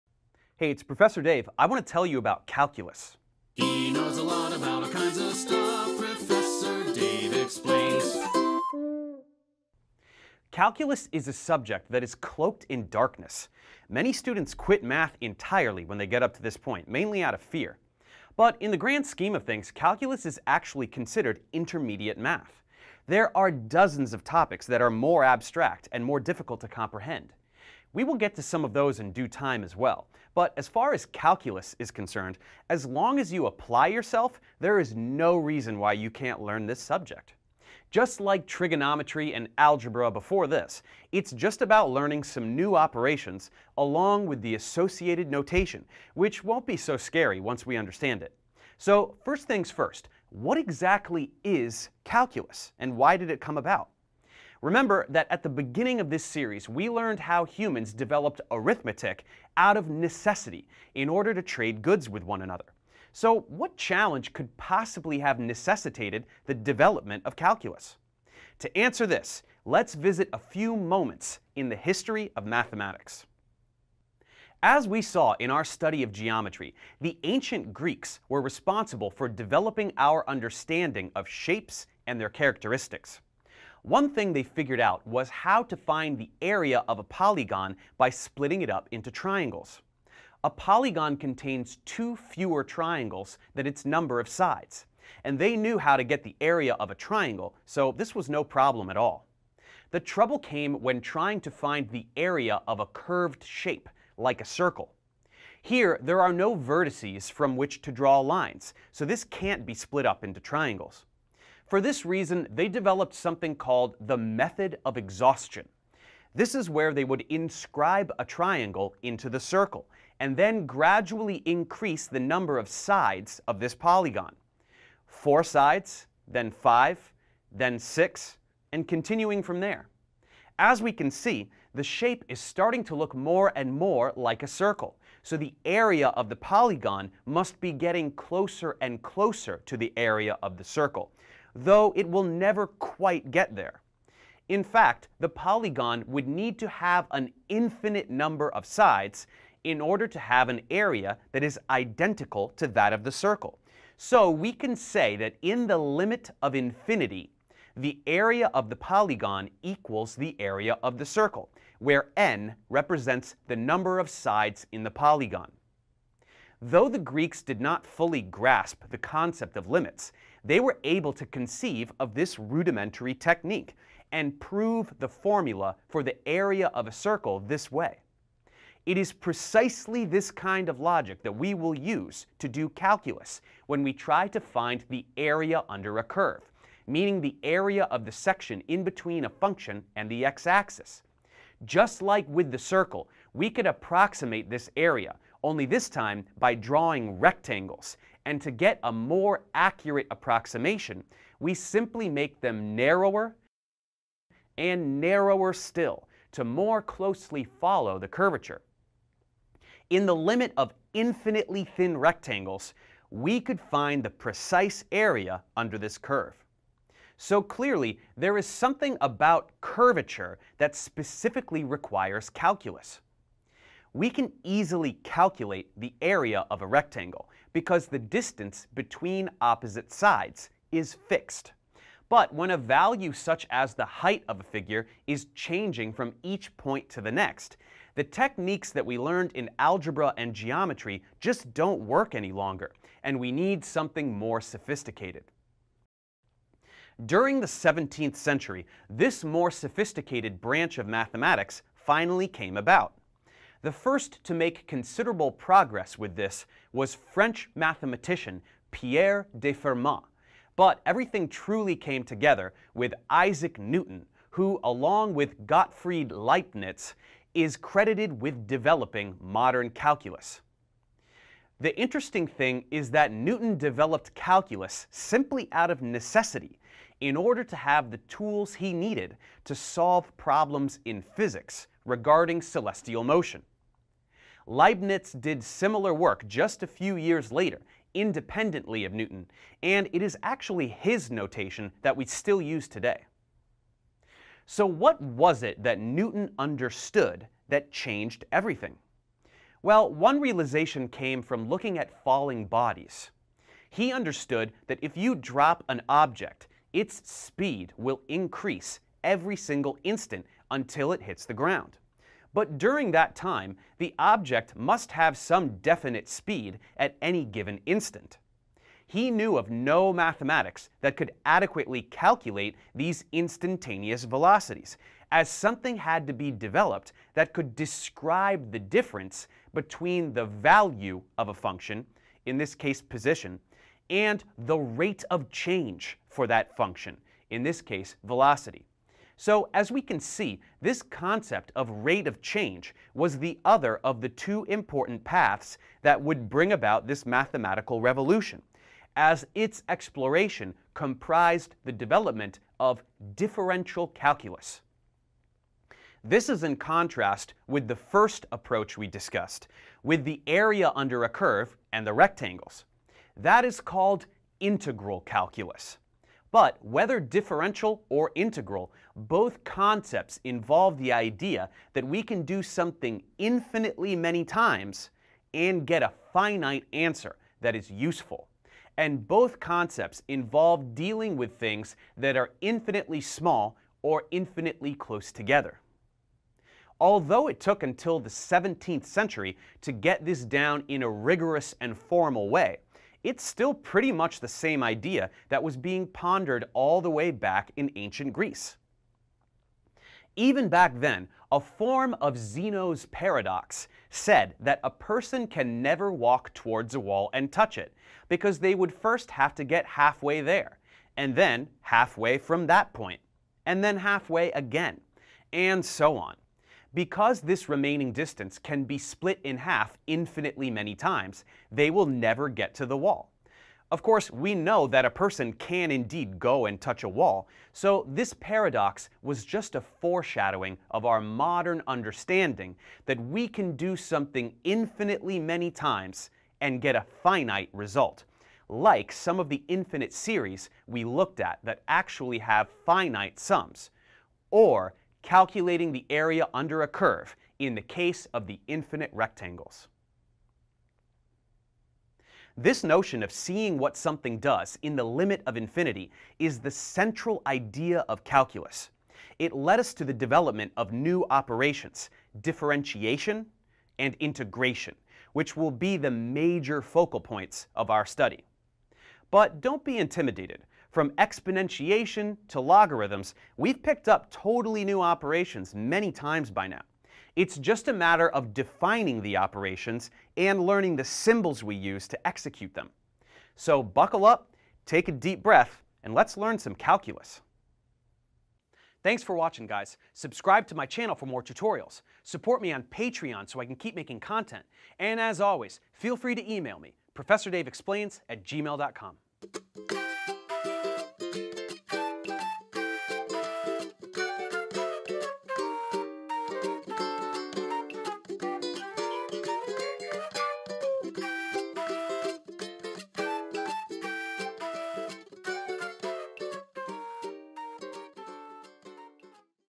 Introduction to Calculus: The Greeks, Newton, and Leibniz
You've been dreading this for a long time, but there's no getting around it! Once we wrap up algebra and trigonometry, it's time to start learning calculus. Don't worry, it's not as bad as you've heard! We just have to learn a couple of new operations and algorithms, but we've done that so many times before! Before we dive into all that, let's learn a little bit about what calculus is and why it came about.